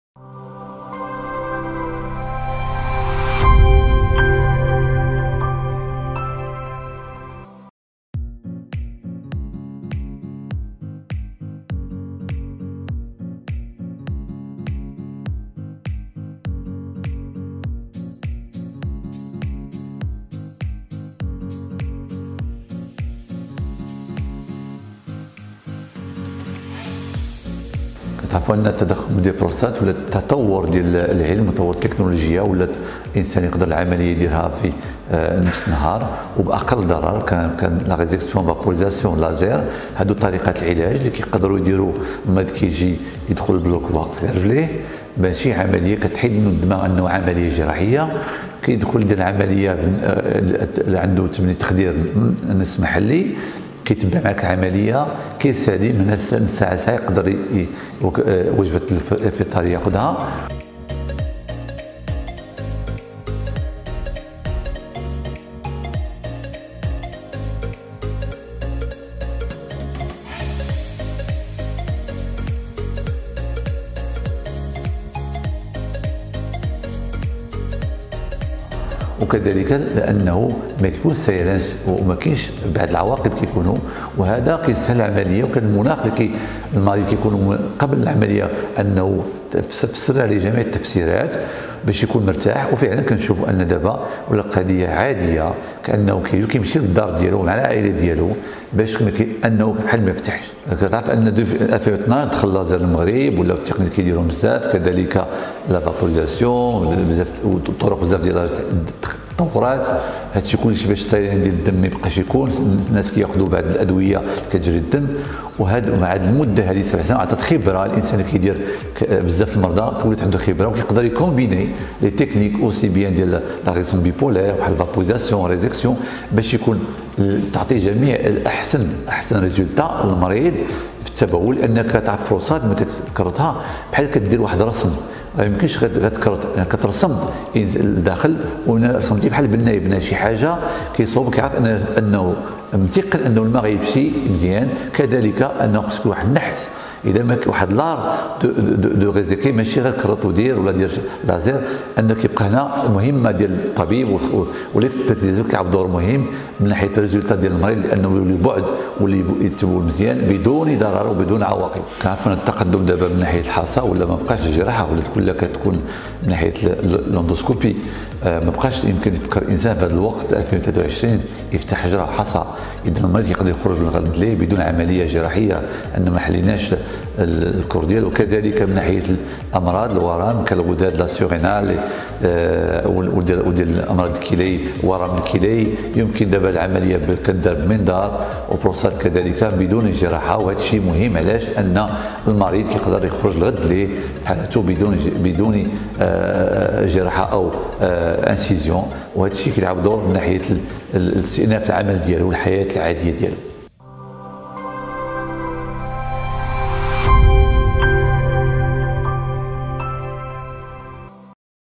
البروفسور ربيع يحمل بشرى لمرضى البروستات.. عملية بدون جراحة في أقل من عشرة دقائق
# البروستاتا
# بروستات